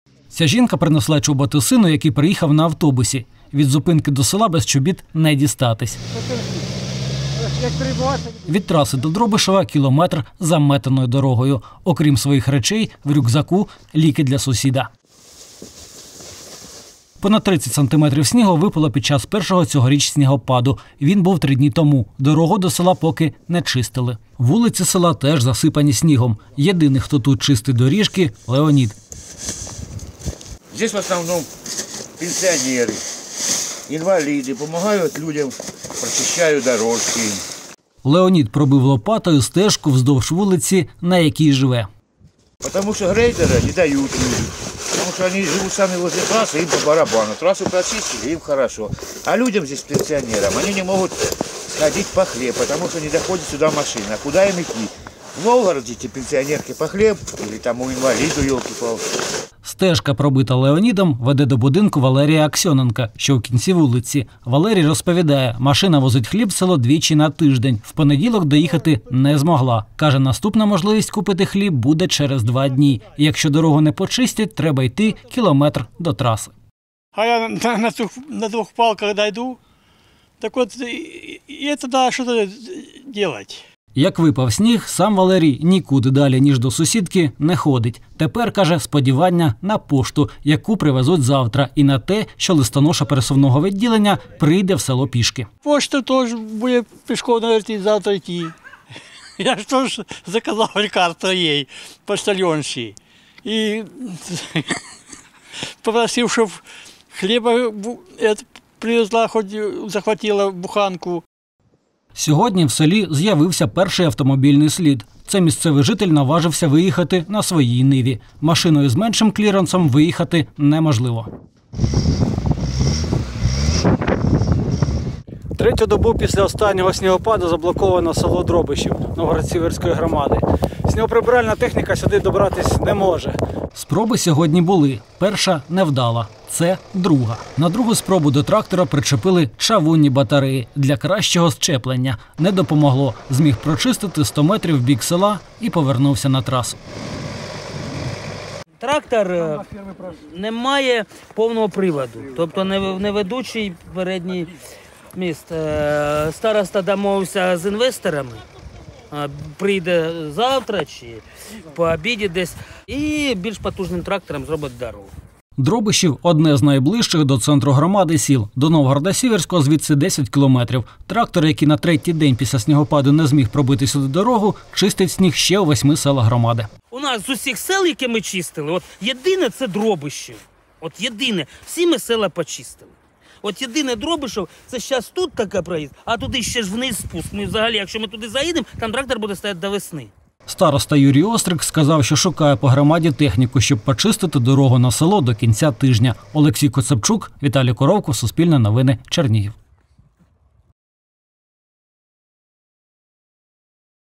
Три доби заблоковані снігом: у селі Дробишів на Чернігівщині не можуть прочистити дорогу
Три доби після снігопаду не можуть прочистити дорогу до села Дробишів на Чернігівщині. Село одне найближчих до центра громади Новгорода-Сіверського, залишається відрізаним. Трактор, який відправили почистити дорогу — не зміг доїхати і повернувся назад. Місцеві жителі залишаються без підвозу хліба, а вулиці в селу чистять лопатами.